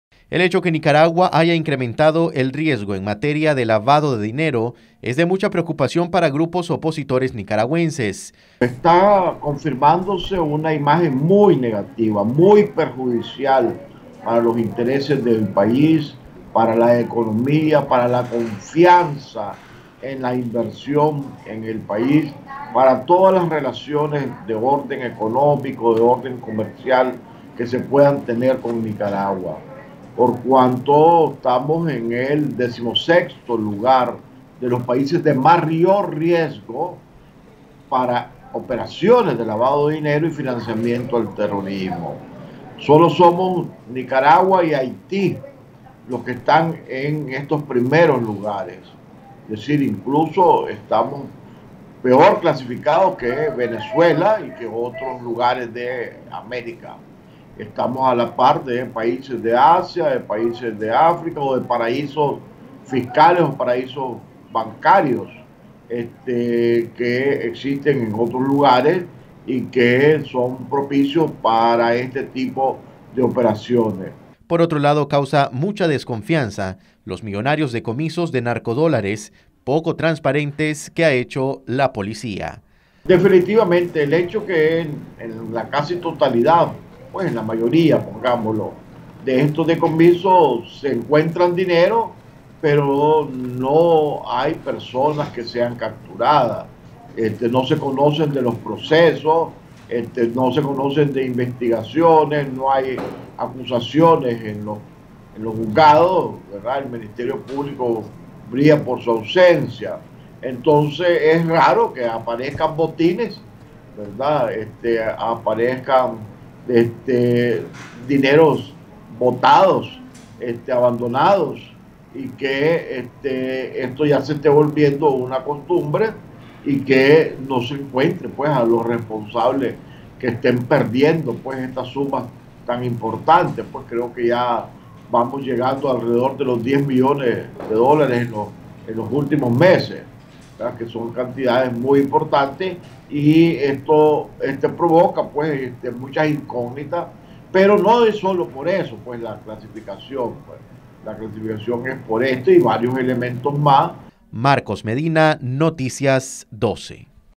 Nicaragua con más alto nivel de riesgo de lavado de dinero según índice de Basilea
Índice de Basilea ubica a Nicaragua en el más alto nivel de riesgo de lavado de dinero.